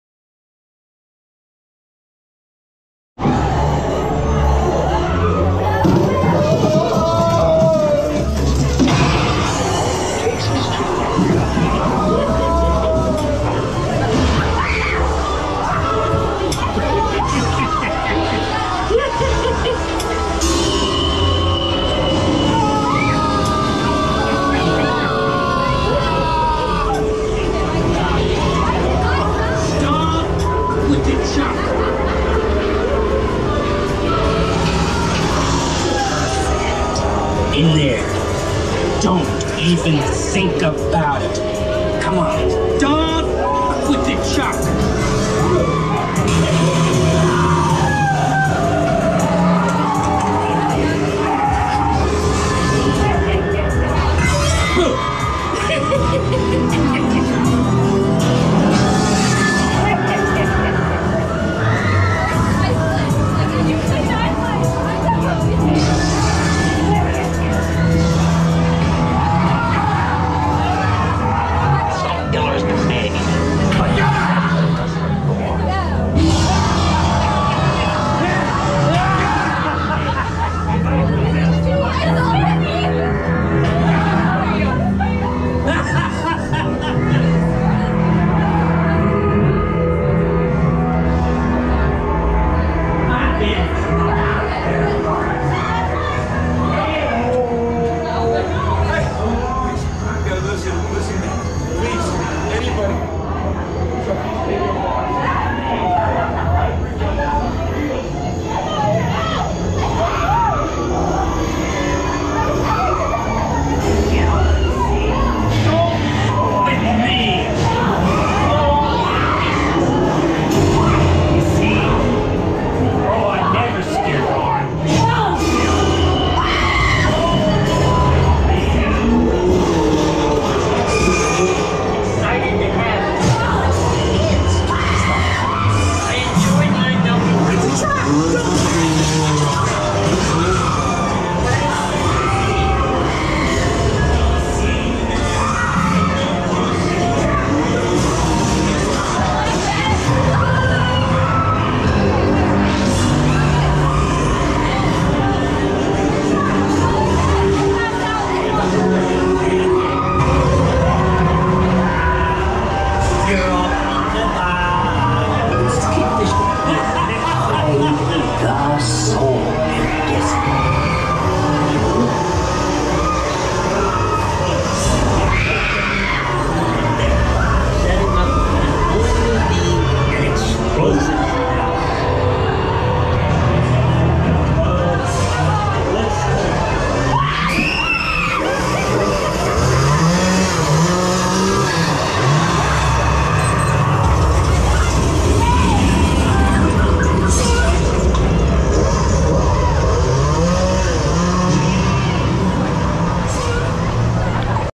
Uncover Chucky's Ultimate Kill Count | Halloween Horror Nights
Get a front row seat for Chucky's ultimate kill count at Halloween Horror Nights! Watch this exclusive POV video to see all the horror and mayhem unfold. Hold on tight and get ready to scream!